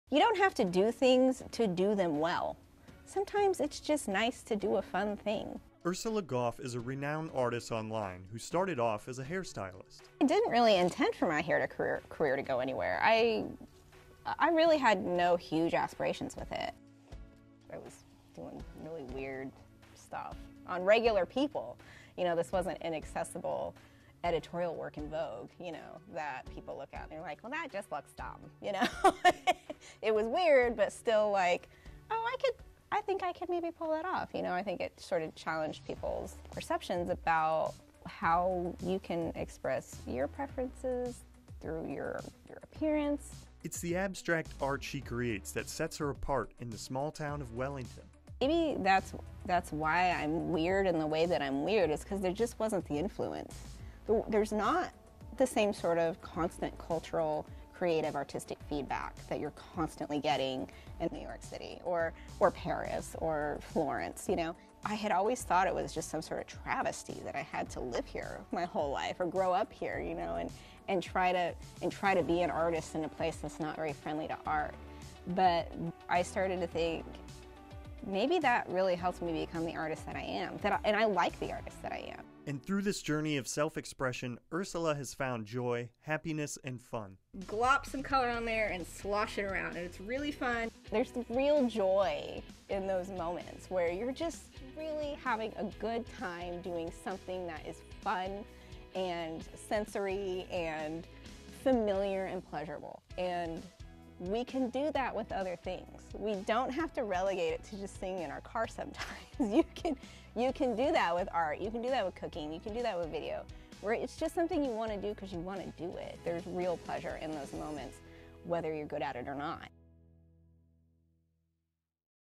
Ursula
An eccentric artist in a small Kansan town reflects on what makes her work unique.